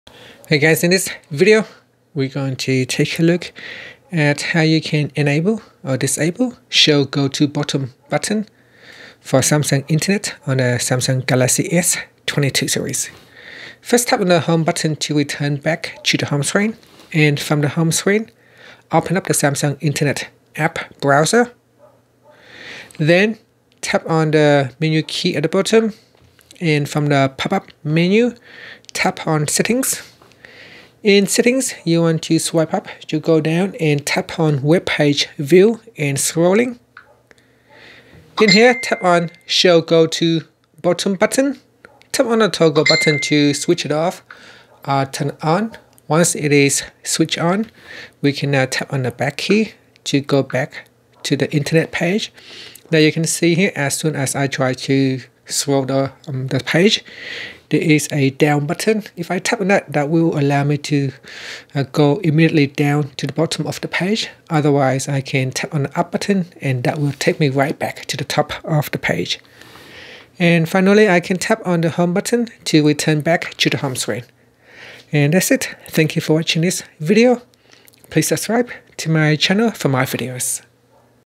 Galaxy S22/S22+/Ultra: How to Enable/Disable Show Go to Bottom Button For Samsung Internet
Learn how you can enable/disable Show Go to Bottom Button for Samsung Internet on the Galaxy S22/S22+/S22 Ultra.